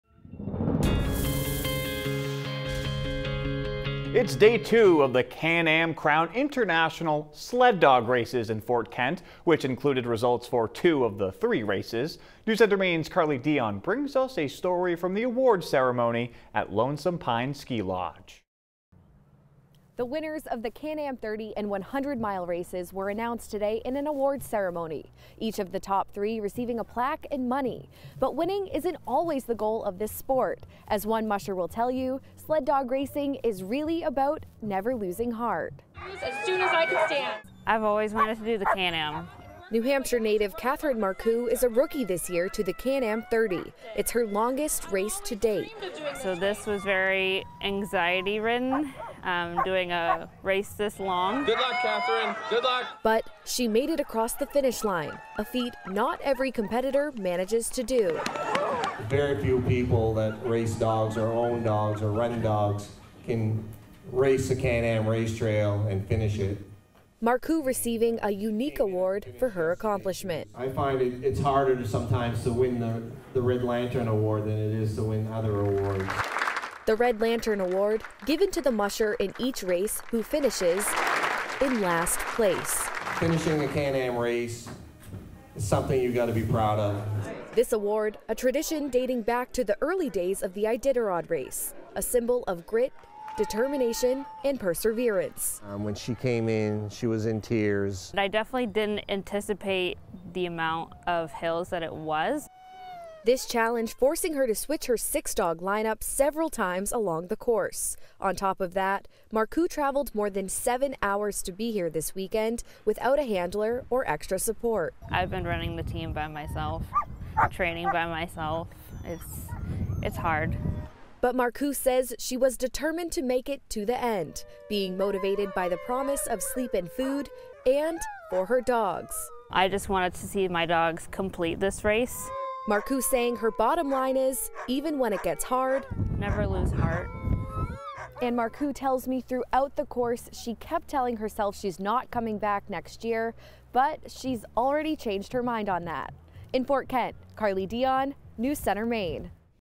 The Can-Am Crown International Sled Dog Race is about more than just winning
"Very few people that race dogs or own dogs or run dogs can race a Can-Am race trail and finish it," Race President Dennis Cyr said.